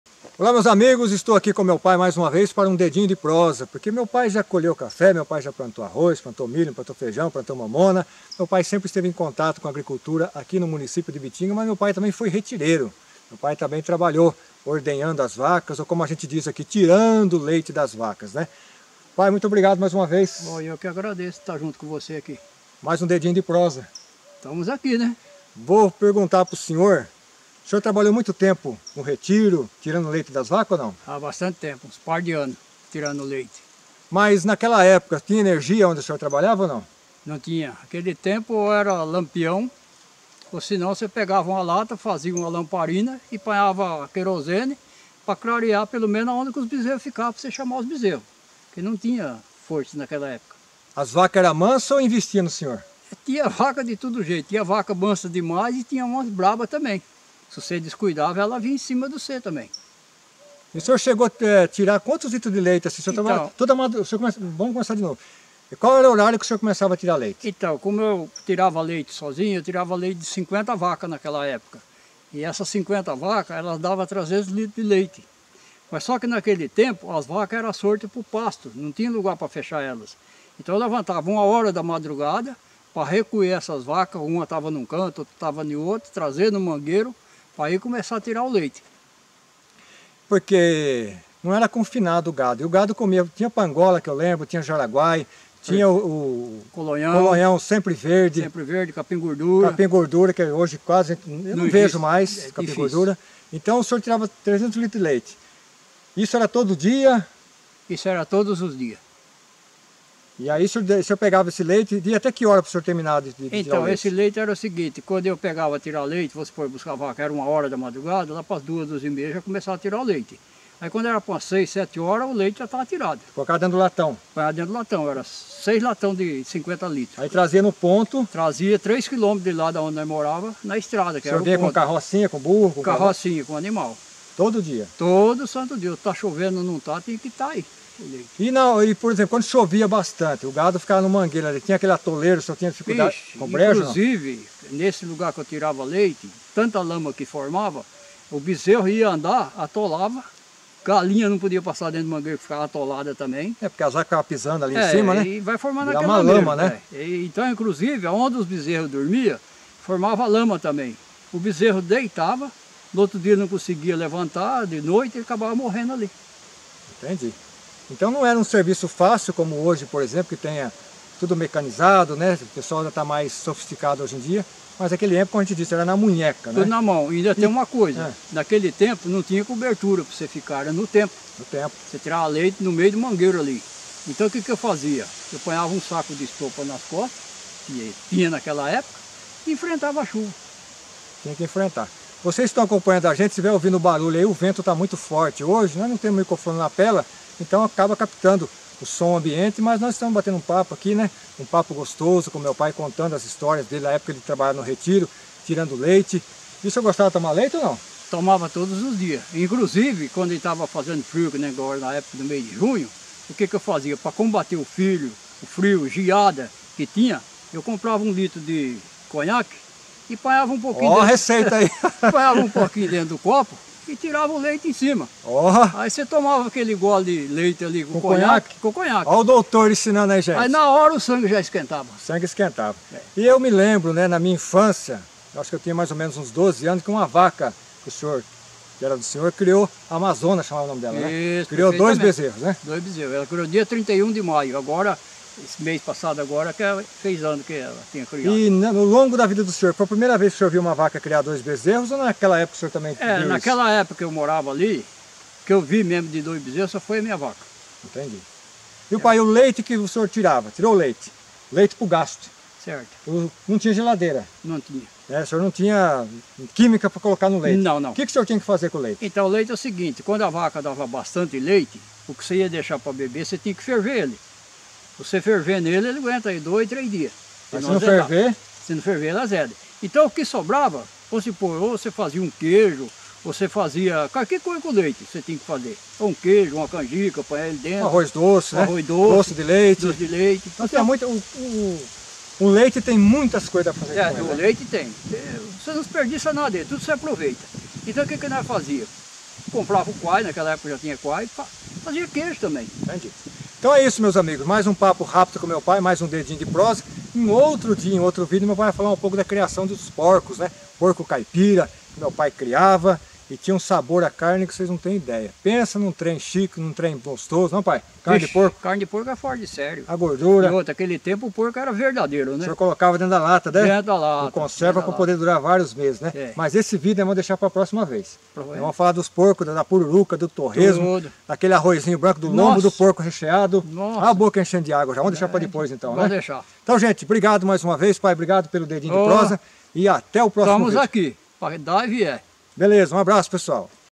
MAIS UM DEDINHO DE PROSA COM O MEU PAI
NESTE VIDIO O MEU PAI CONTA UM POUCO DA EPOCA QUE ELE TRABALHAVA TIRANDO LEITE DAS VACAS